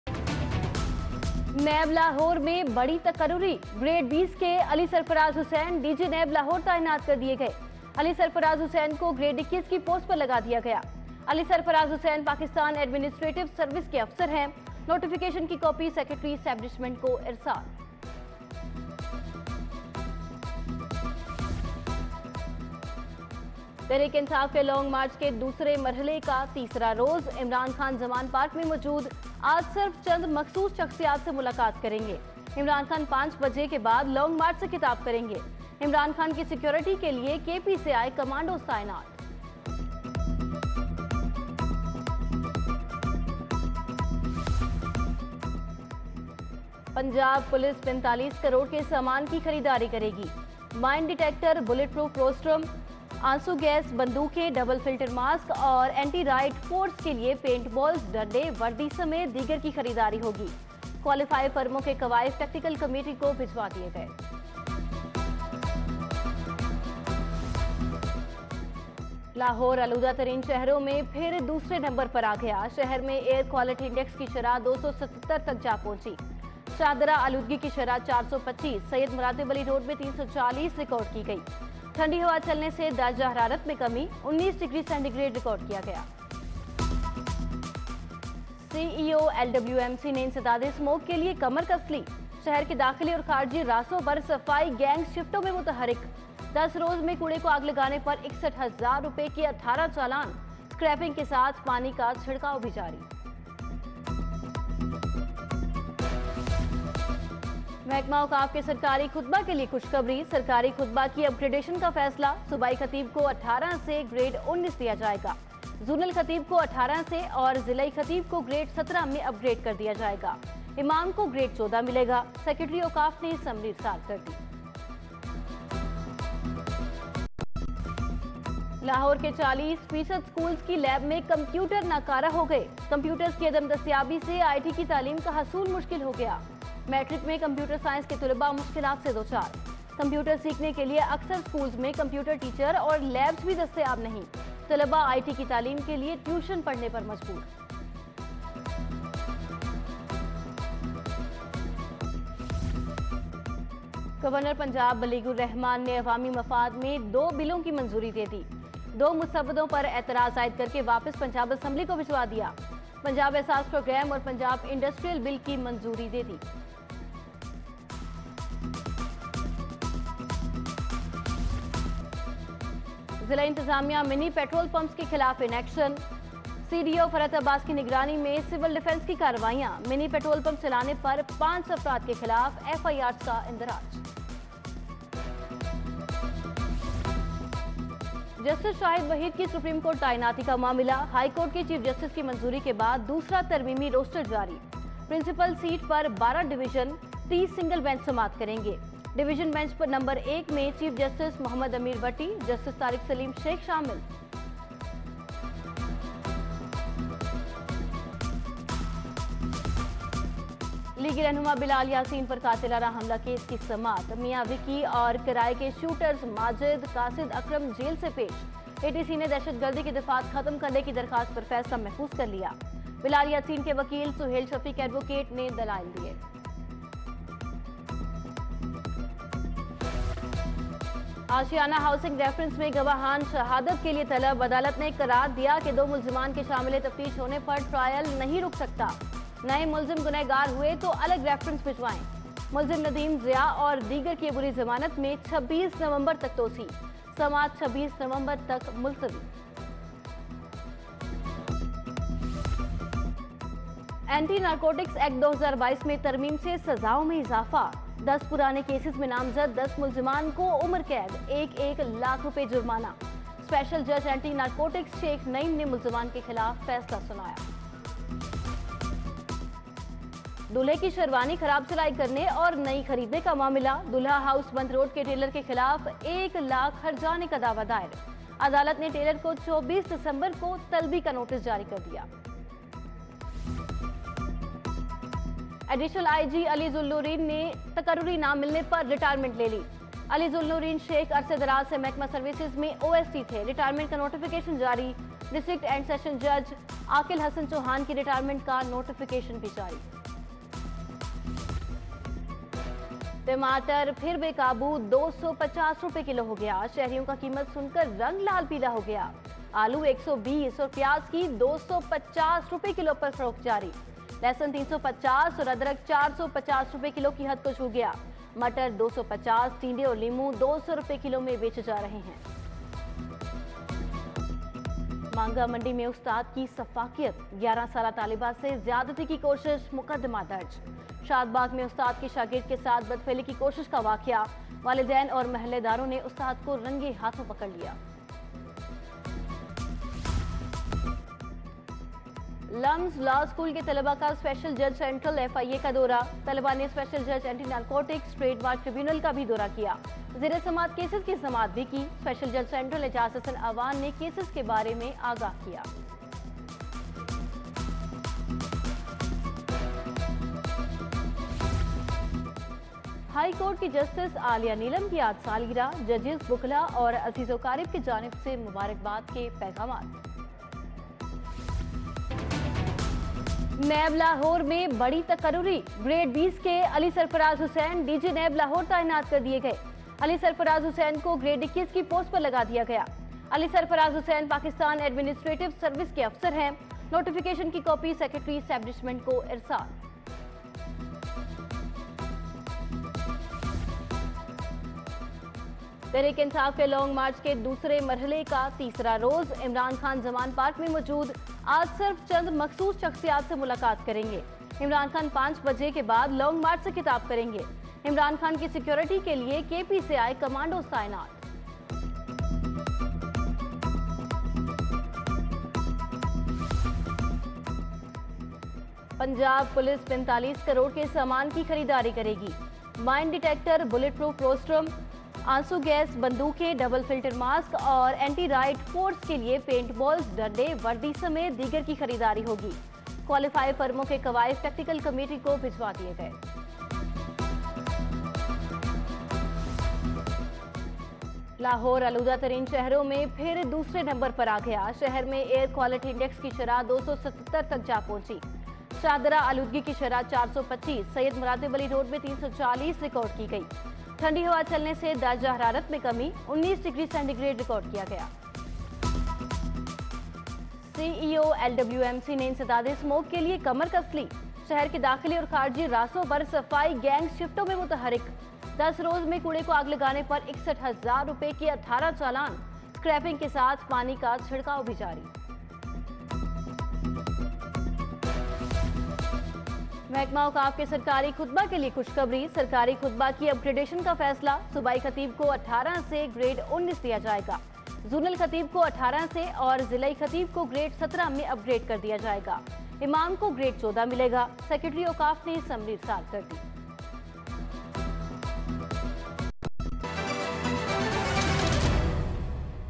عمران خان کیلئے کی پی سے سیکورٹی منگوالی، 60 ہزار کا جرمانہ، صبح 12 بجے کی ہیڈلائنز
12pm News Headlines | 12 Nov 2022 | City 42